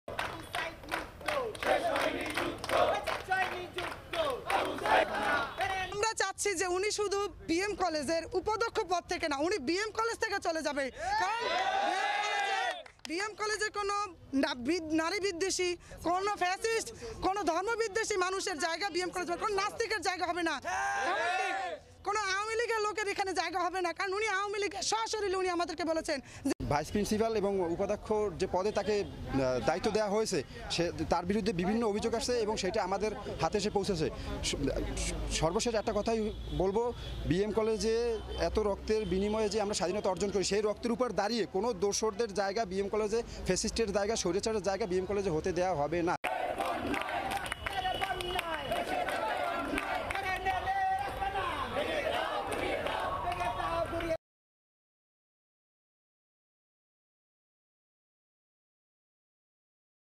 বরিশালে বিএম কলেজের উপাধ্যক্ষের অপসারণের দাবিতে বিক্ষোভ | Barishal | BM College | Student Protest
»» Office Address:
Bangladesh.